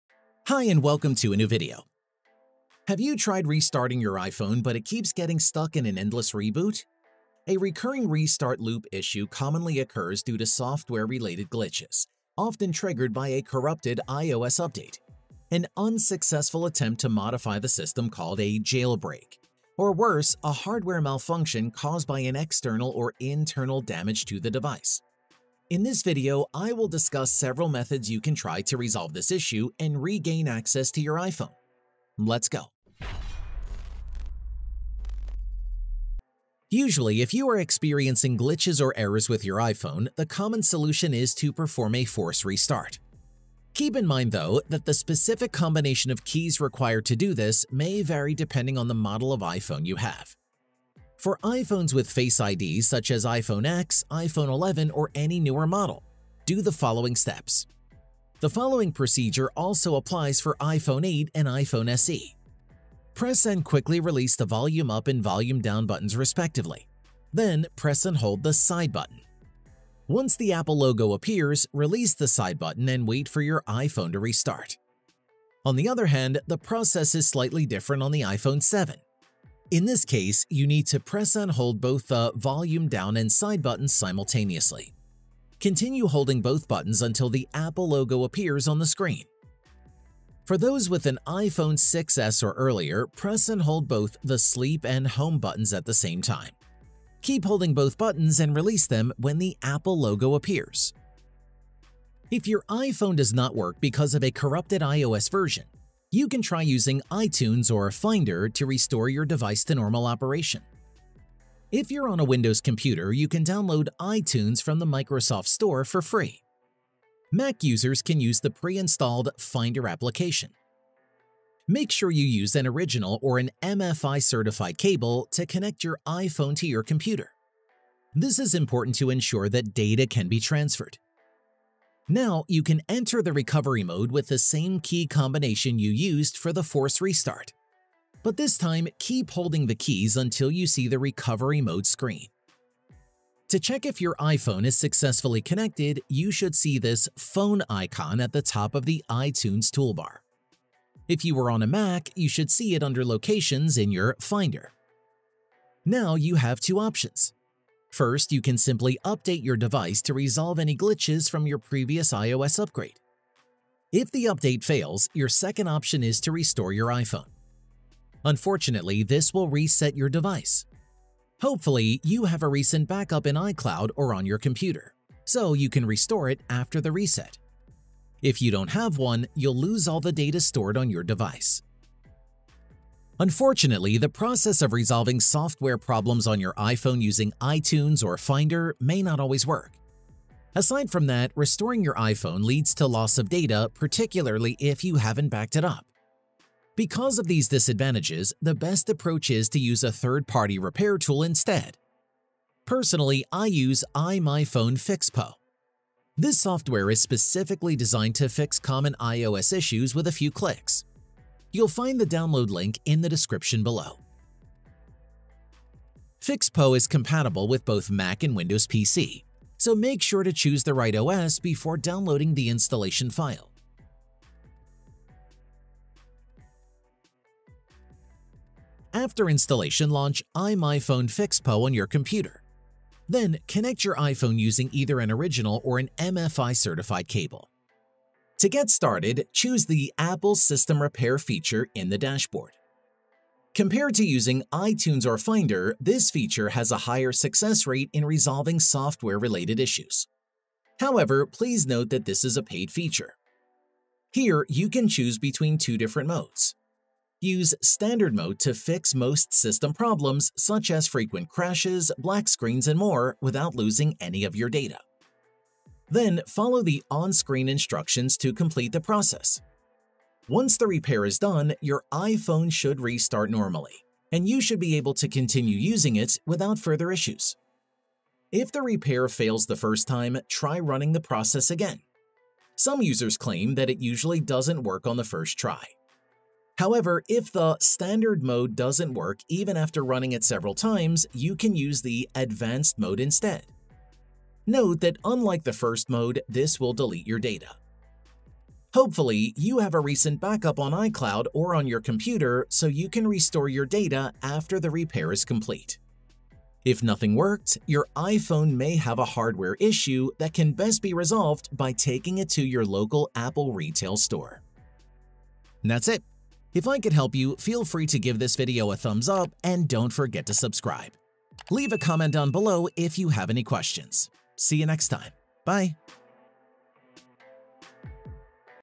Fix iPhone Stuck in Restart Loop/Endless Reboots! [3 Methods]
Have you tried restarting your iPhone, but it keeps getting stuck in an endless reboot cycle? The boot loop is often caused by errors that are mostly software related and can be easily fixed. In this video, I will show you how to fix this problem and regain access to your iPhone. Yours Jigxor

Most important links:
* | Download iMyFone Fixppo

Timestamps:
00:00 | Force Restart
01:41 | Restore with iTunes/Finder
03:02 | Use a Repair Tool

Video: 507